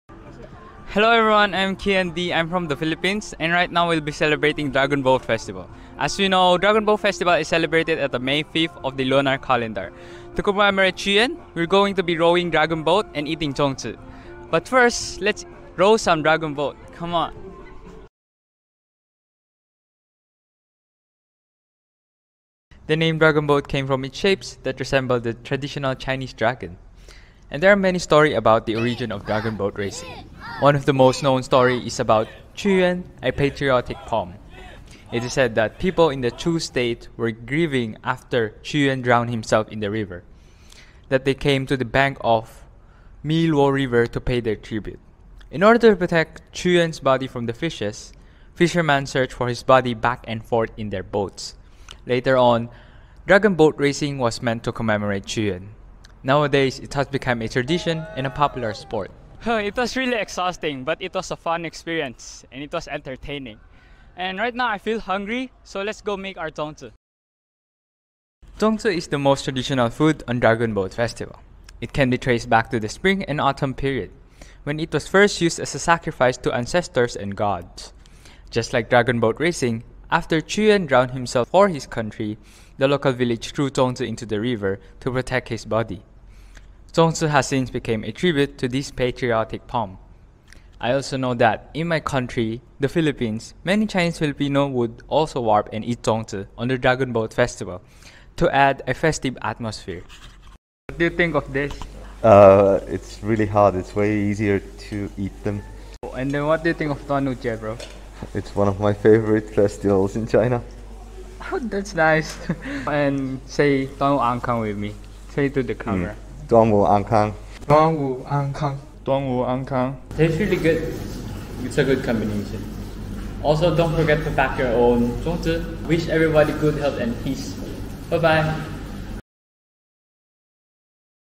International students at Huaqiao University also immerse themselves in festive activities
Dragon Boat Festival is celebrated across China on Monday. International students at Huaqiao University also immerse themselves in festive activities of dragon boat racing and zongzi making. Check this video and take home their blessings!